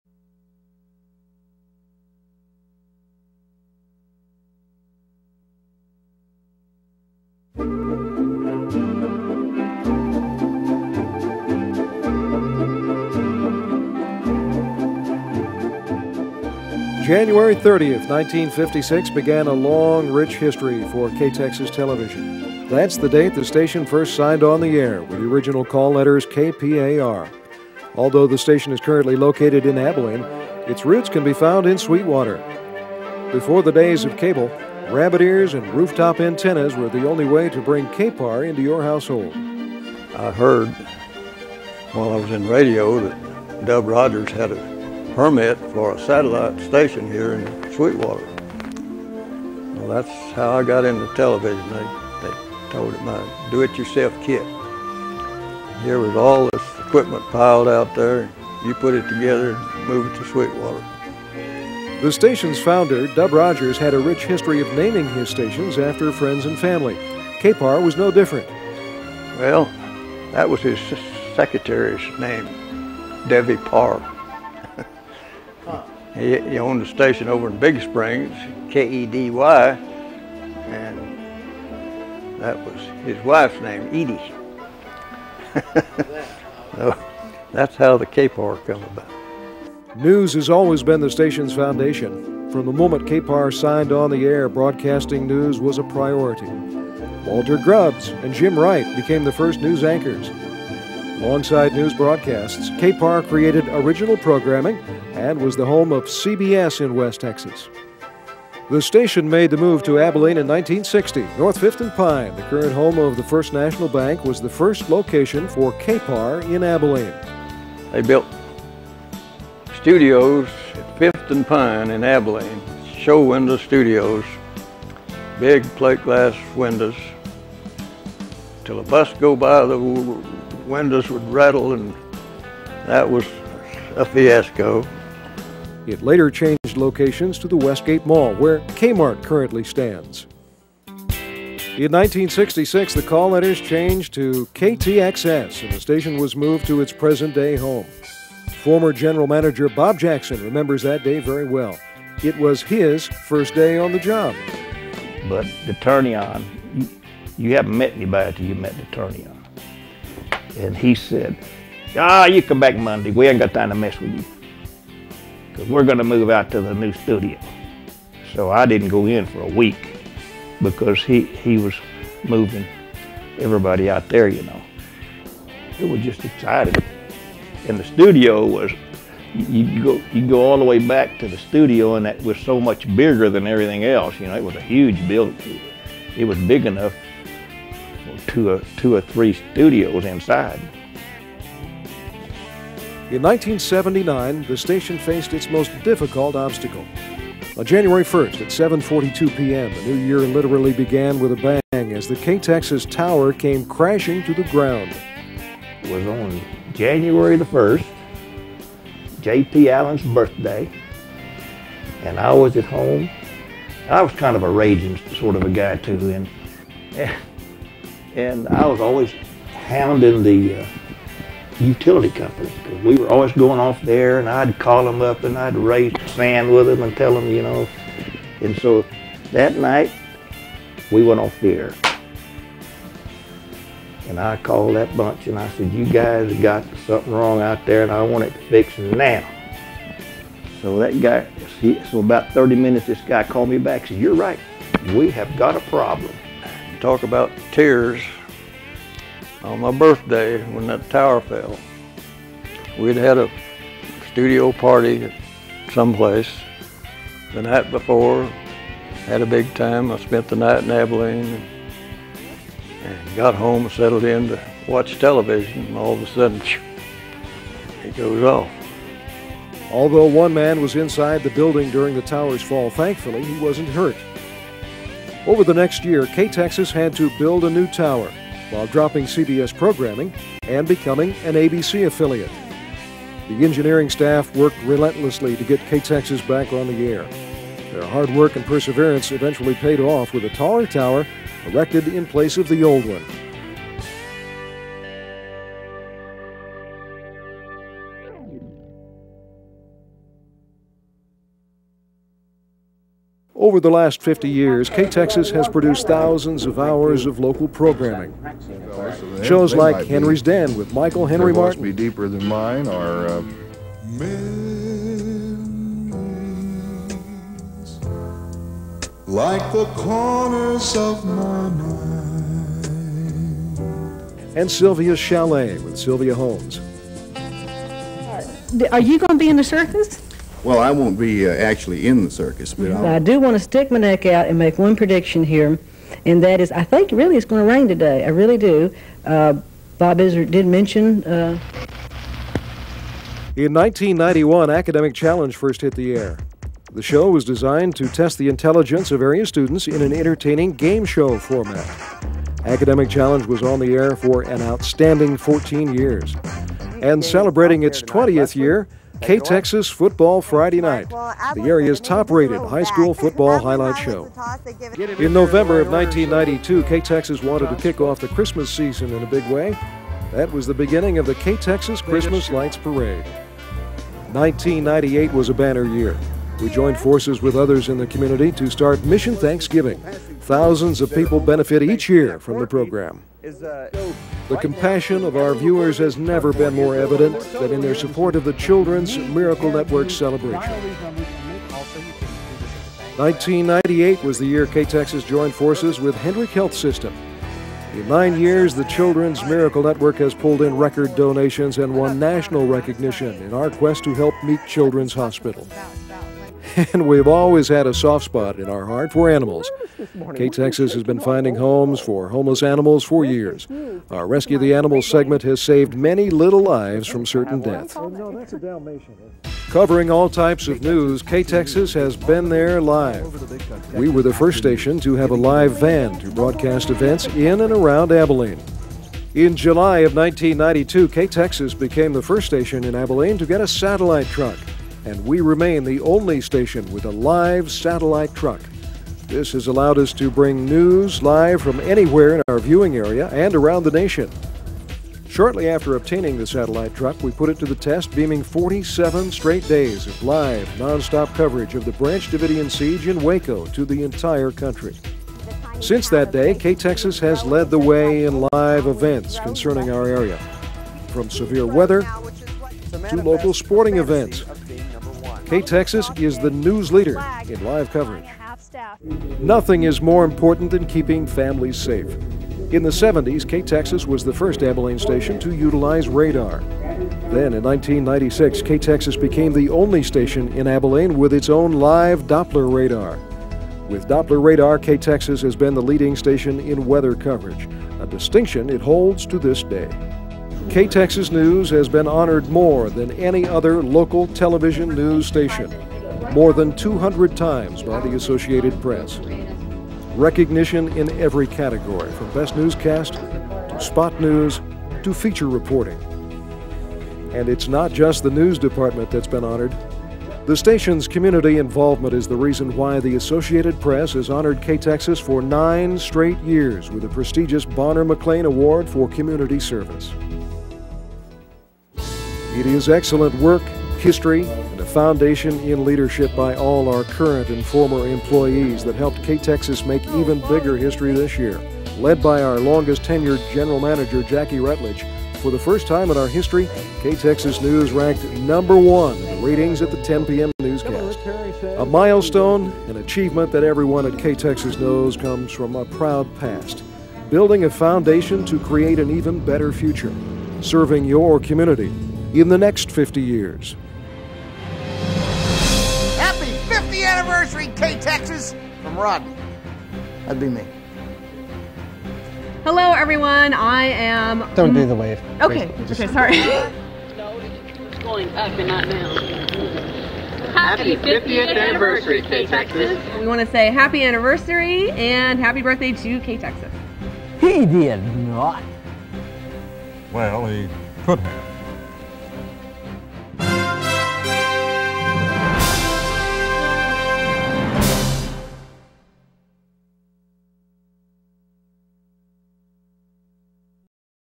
KPAR-TV Celebrates 50th Anniversary
It is now called KTXS-TV located in Abilene, Texas. I went to work there in 1960 and worked there until I moved to the flagship station of Texas Key Broadcasters, KLBK-AM, FM, TV. I landed a job with Ampex Corporation in 1973 and finished my broadcast electronics career with them 37 years later!